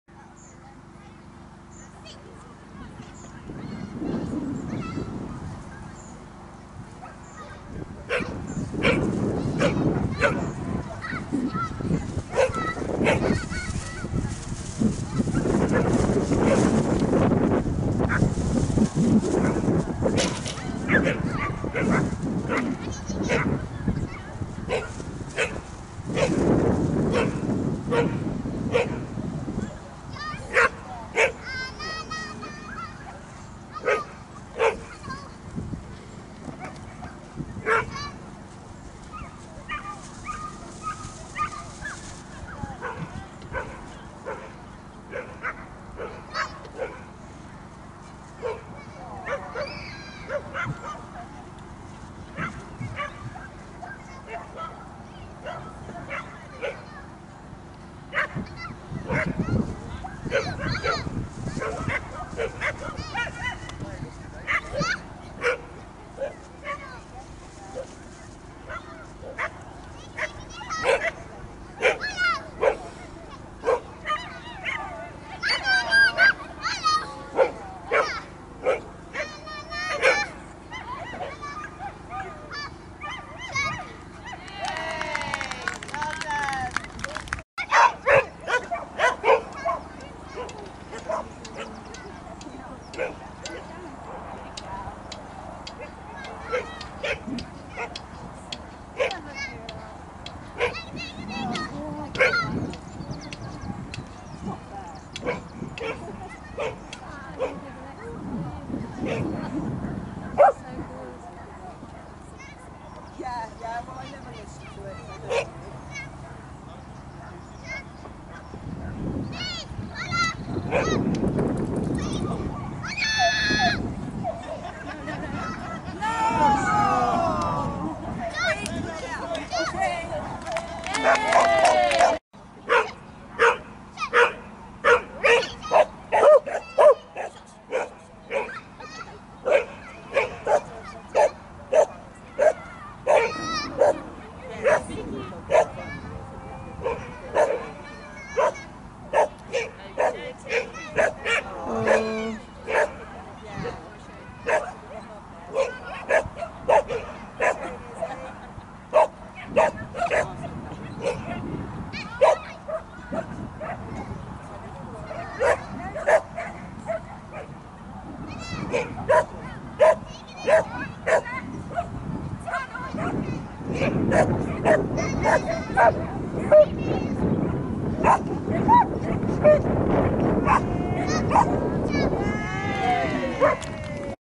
Hoolihounds 15th & 16th June 2019
First KC Show competing in Grade 6-7 classes. Just slightly harder than anysize! Some very difficult courses, a few diva strops, 6 eliminations but also some very good handling,